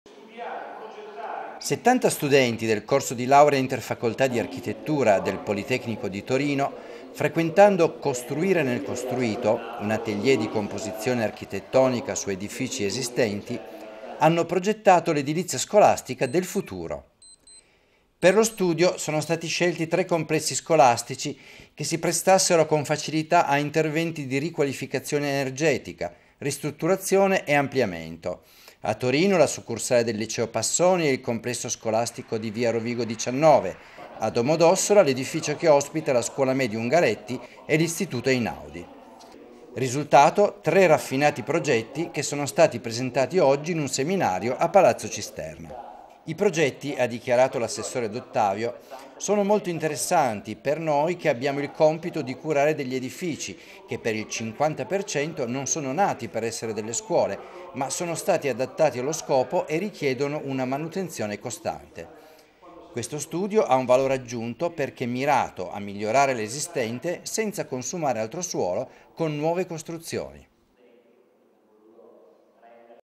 I giovani rinnovano l'edilizia scolastica
Presentati i risultati di "Costruire nel costruito", un'esperienza didattica degli studenti del corso di laurea in architettura del Politecnico torinese. (giugno 2012)